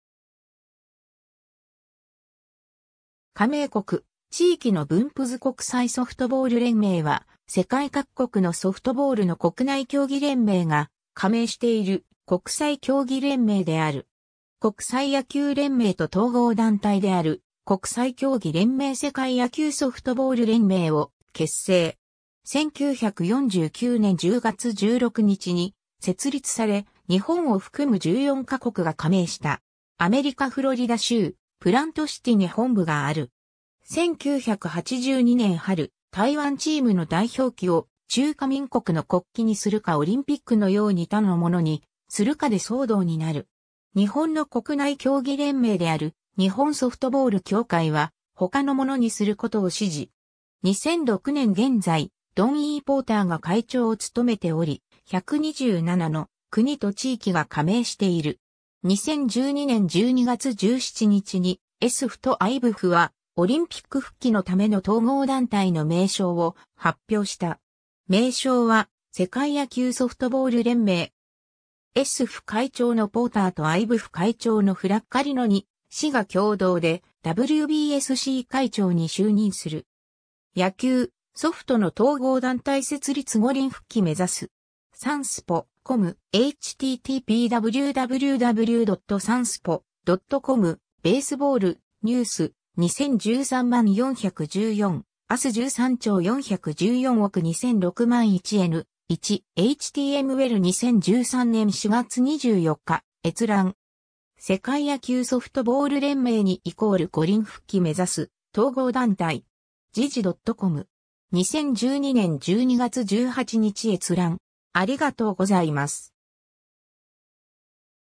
国際ソフトボール連盟
加盟国・地域の分布図 国際ソフトボール連盟（こくさいソフトボールれんめい、英："I"nternational "S"oftball "F"ederation、略：ISF）は、世界各国のソフトボールの国内競技連盟が加盟している国際競技連盟である。
 国際野球連盟 (IBAF) と統合団体である国際競技連盟世界野球ソフトボール連盟 (WBSC) を結成。
 1949年10月16日に設立され、日本を含む14ヶ国が加盟した。
アメリカ・フロリダ州プラントシティに本部がある。
1982年春、台湾チームの代表旗を中華民国の国旗にするかオリンピックの様に他のものにするかで騒動になる。
日本の国内競技連盟である日本ソフトボール協会は他のものにすることを支持。
2006年現在、ドン・E・ポーターが会長を務めており、127の国と地域が加盟している。
 2012年12月17日にISFとIBAFは、オリンピック復帰のための統合団体の名称を発表した。
名称は世界野球ソフトボール連盟 (World Baseball Softball Confederation) 。
ISF会長のポーターとIBAF会長のフラッカリの二氏が共同でWBSC会長に就任する。
^ “野球・ソフトの統合団体設立　五輪復帰目指す”. SANSPO.COM. (2013年4月14日). 2013年4月24日閲覧。
 ^ “「世界野球ソフトボール連盟」に＝五輪復帰目指す統合団体”. 時事ドットコム (2012年12月18日). 2012年12月18日閲覧。